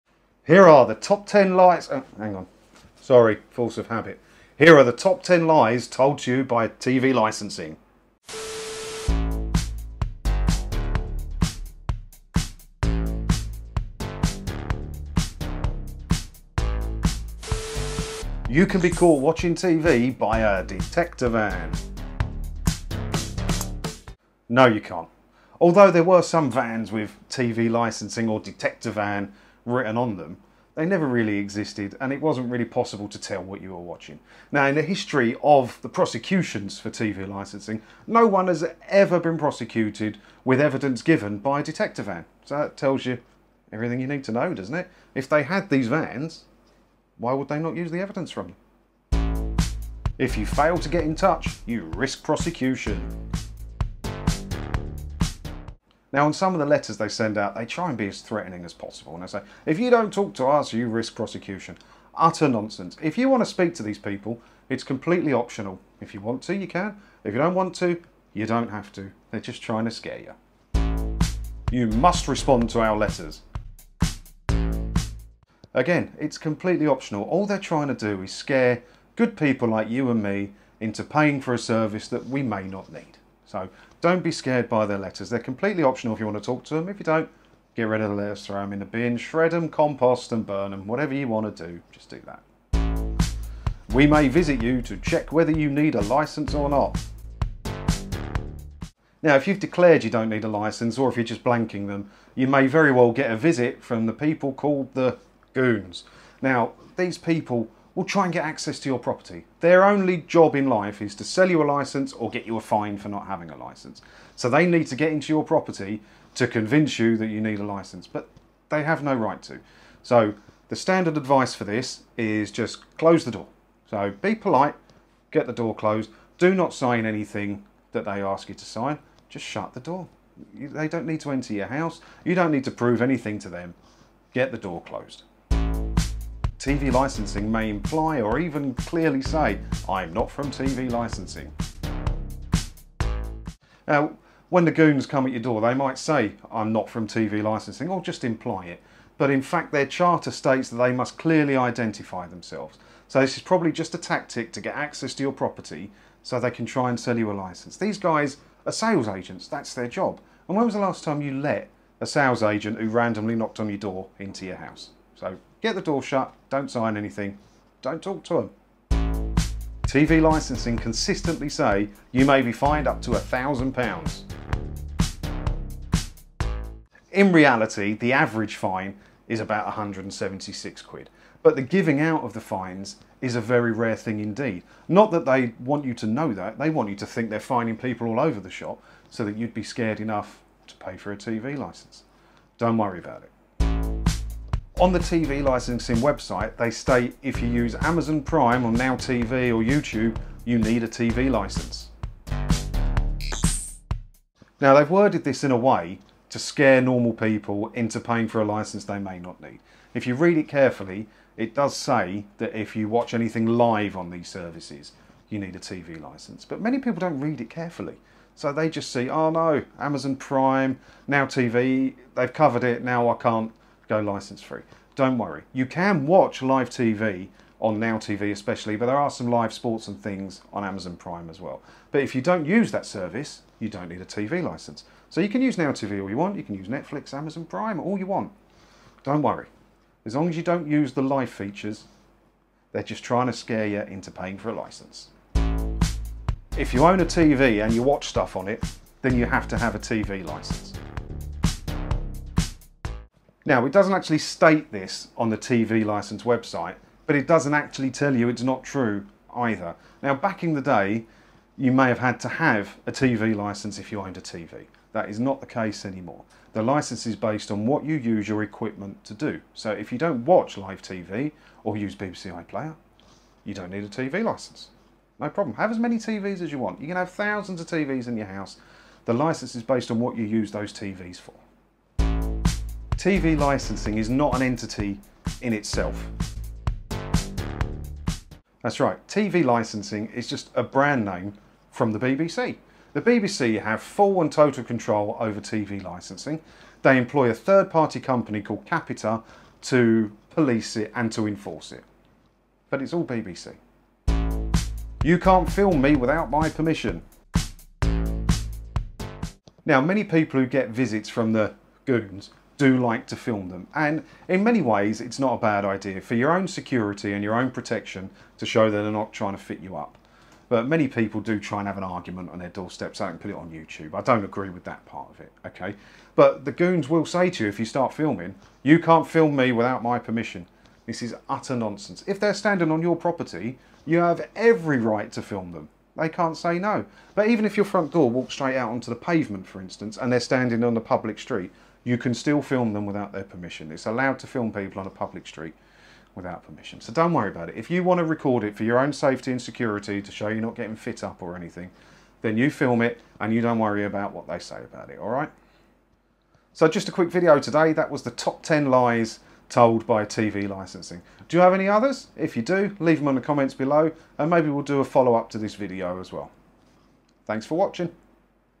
The Top 10 TV Licence Lies
Here are the top ten lies told abut the TV Licence. Many of these were suggestions from the people over So thank you for the help on this.

If you would like to support what I do, take a look here for all the ways you can

Best deals on video doorbells